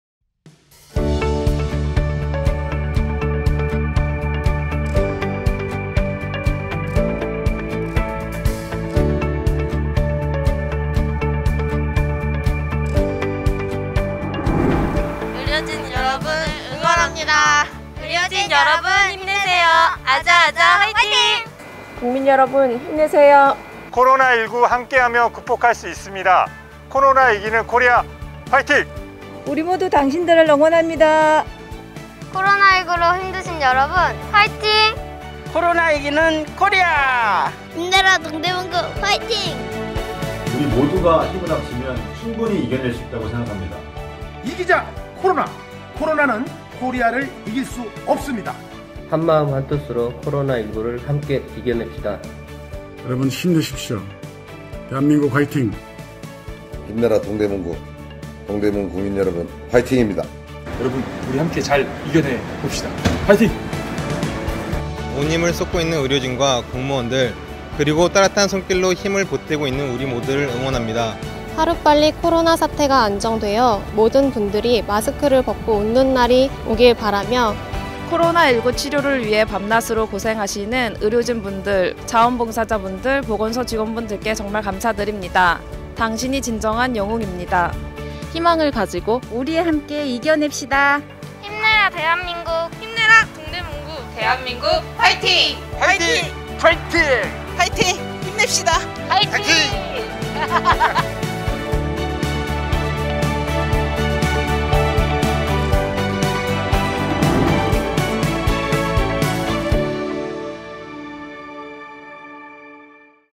동대문구 코로나19 극복 응원 릴레이(재능기부 제작)
코로나19 극복 응원 릴레이 힘내라! 동대문구
(본 영상은 참여자 모두의 재능기부로 만들어 졌습니다!)